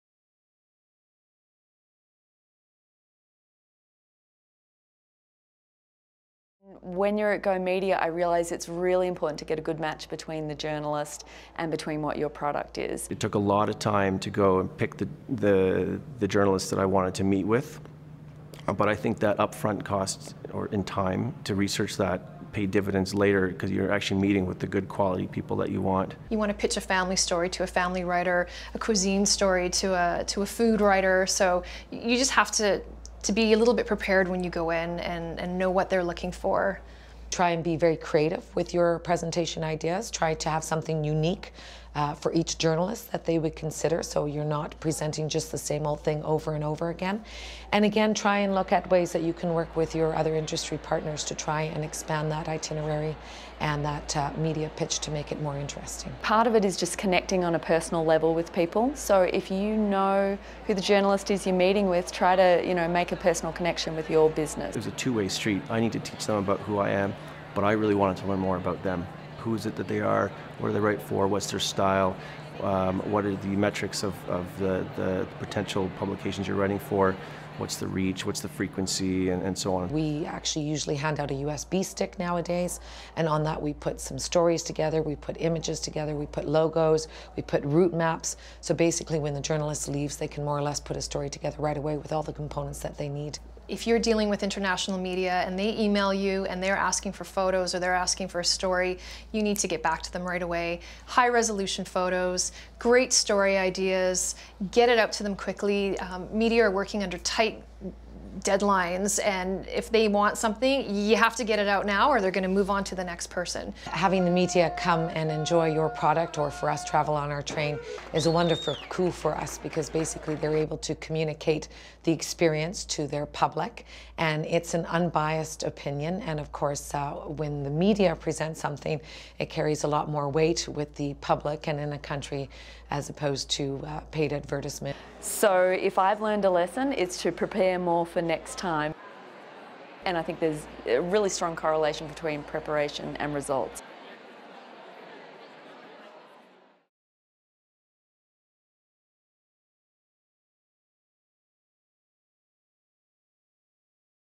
Tips from attendees for a successful GoMedia
Tourism businesses who have attended GoMedia provide tips for a successful experience and event.